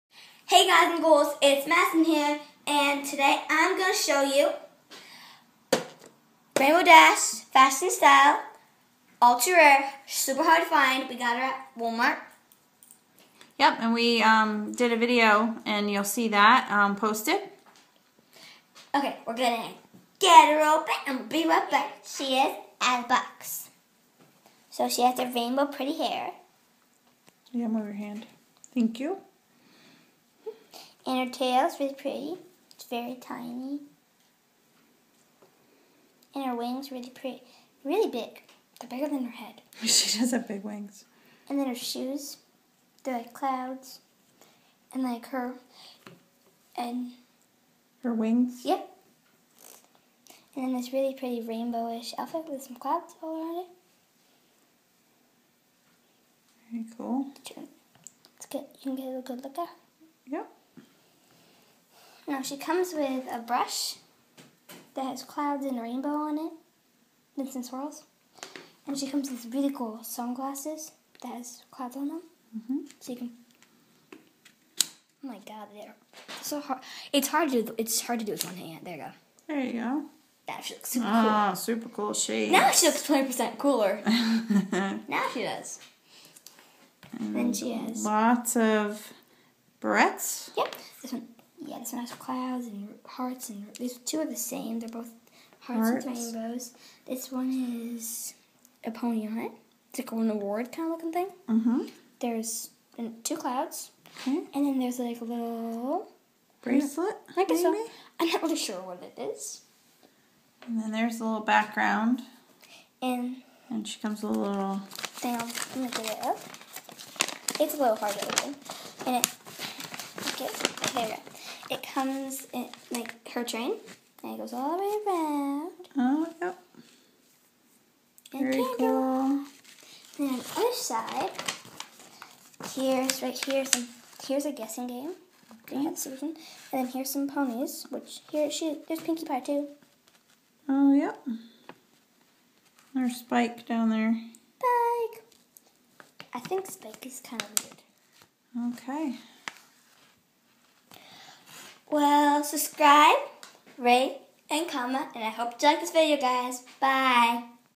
My Little Pony Rainbow Dash Review
I got Rainbow Dash with my allowance money today. Here is my review.